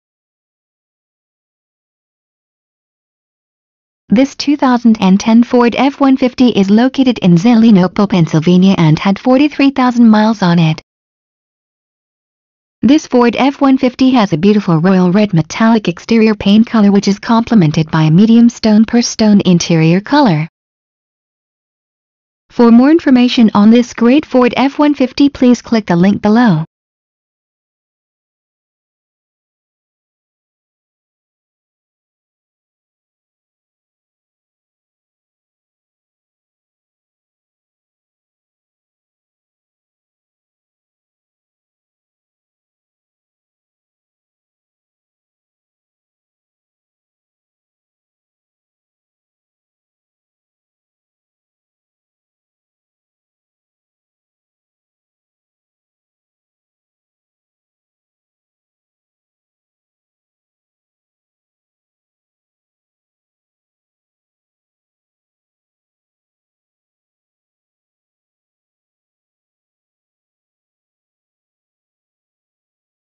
2010 Ford F-150 Pittsburgh Wexford Cranberry PA
Baierl Ford
540 S Main St, Zelienople PA 16063

Price: $ 25,000
Mileage: 43,000
Trim: XLT
Ext. Color: Royal Red Metallic
Int. Color: Medium Stone/Stone
Transmission: Automatic
Engine: 5.4L V8 24V MPFI SOHC Flexible Fuel
Doors: 4
Stock #: ZD374A
VIN: 1FTFX1EV9AFB37906
Description: